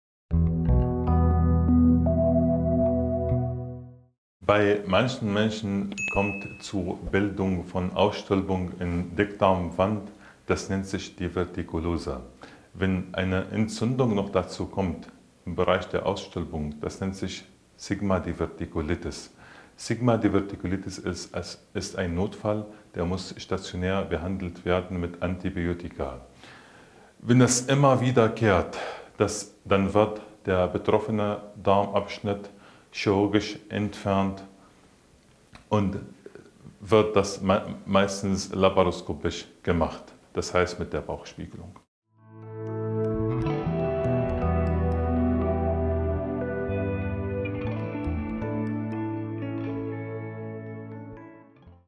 Was ist eine Divertikulitis? - Medizin ABC | Asklepios
Eine Divertikulitis bringt unschöne Beschwerden mit sich. Es kommt zu plötzlichen Schmerzen im Unterbauch, manchmal begleitet von leichtem Fieber und Übelkeit. Erbrechen gehört zu den seltenen Symptomen. Einige Patienten berichten außerdem über Eiter, Schleim oder Blut im Stuhl. Wenn durch die Divertikulitis eine krankhafte Verbindung zwischen Darm und Blase entstanden ist, kann auch das Wasserlassen schmerzhaft sein. Abd Alraheem ist Departmentleiter der Allgemein- und Viszeralchirurgie an der Asklepios Klinik Sebnitz und erklärt, was eine Divertikulitis ist.